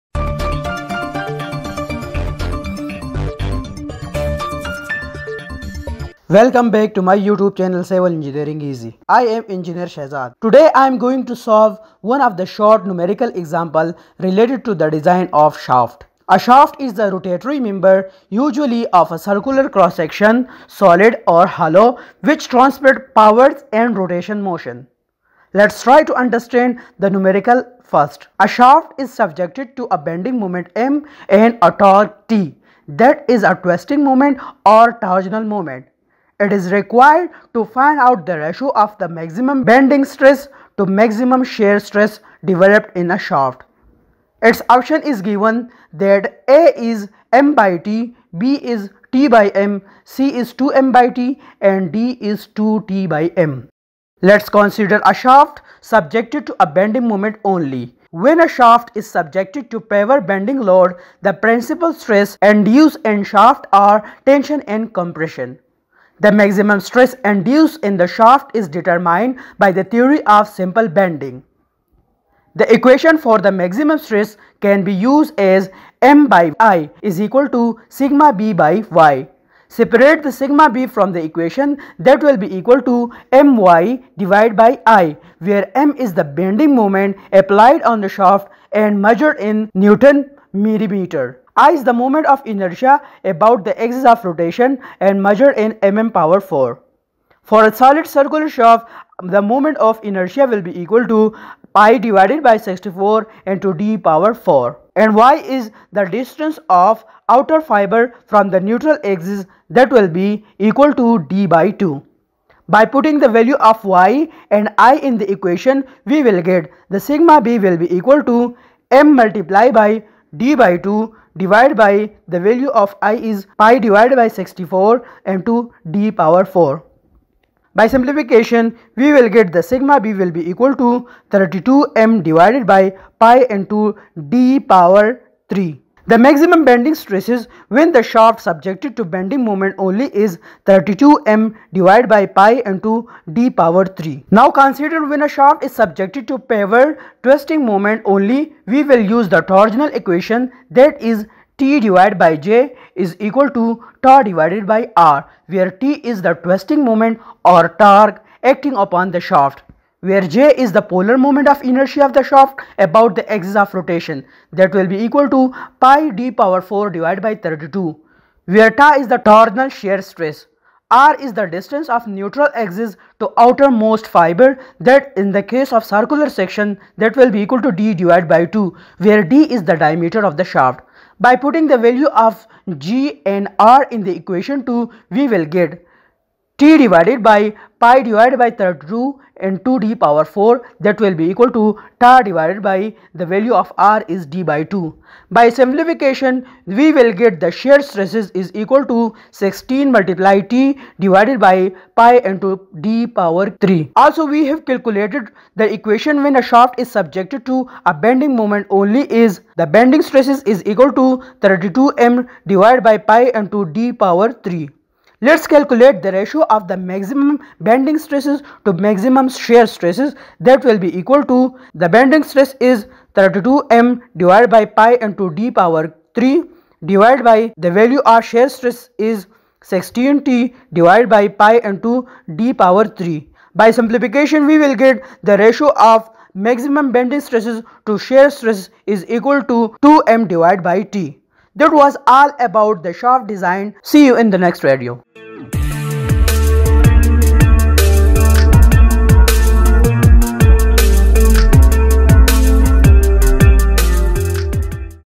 Bending and shear stresses calculation in Shaft | Shaft Design | Mechanics of Solid
In this short video, you will learn to calculated the bending and shear stresses calculation when a shaft is subjected to bending moment and twisting moment.
After the calculation of maximum bending and stress and shear stress in shaft the ratio of maximum bending and shear stress are calculated.